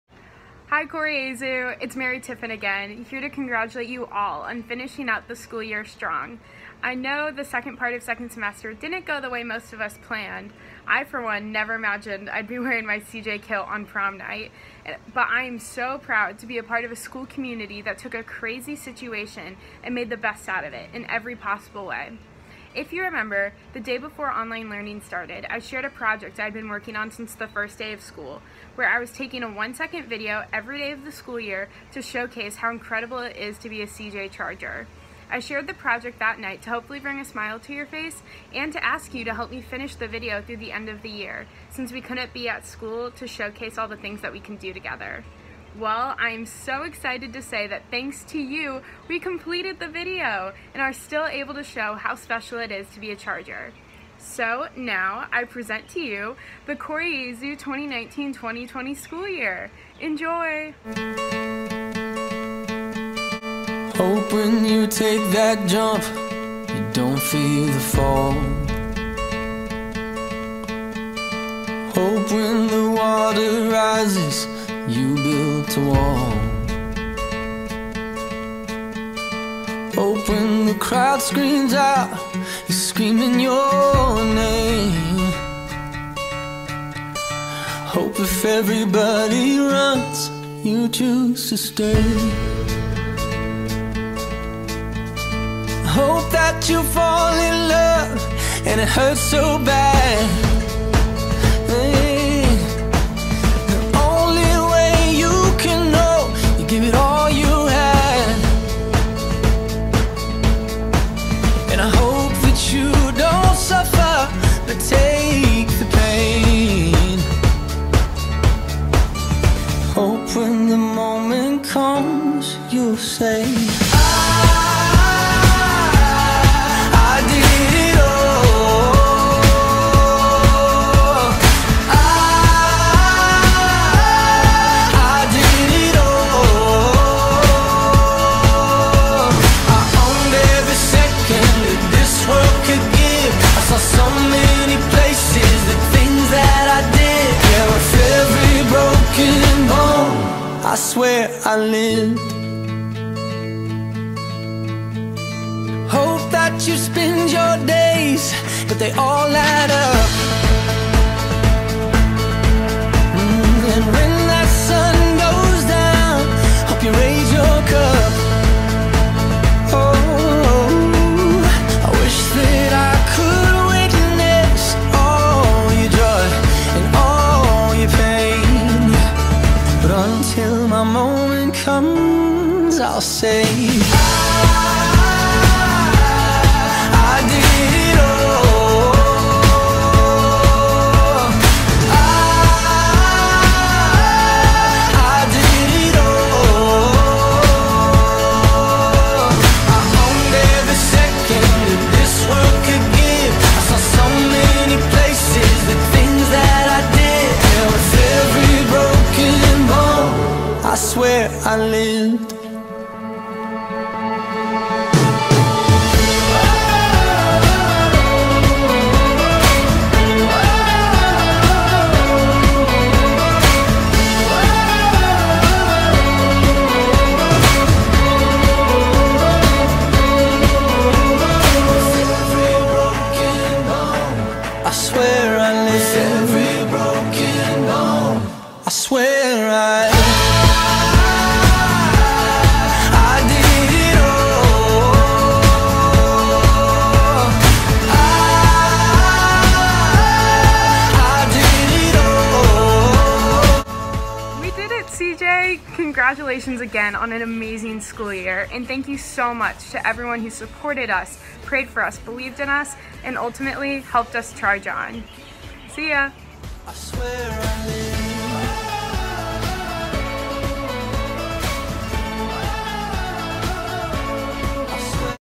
Cor Jesu Academy - FULL 2019-2020 School Year
Cor Jesu Academy senior, Mary Tiffin '20, spent the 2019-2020 school year taking a one-second video each day of the year to showcase how special it is to be a Cor Jesu Charger. Due to the coronavirus (COVID-19) Cor Jesu began online learning in mid-March, so Mary called on the CJ community to send in their own videos to help her finish the product through the end of the school year. We are proud to share Mary's completed video project - a recap of the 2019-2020 school year. It may have had an unexpected twist, but the CJA community found a way, as always, to ChargeOn. Enjoy!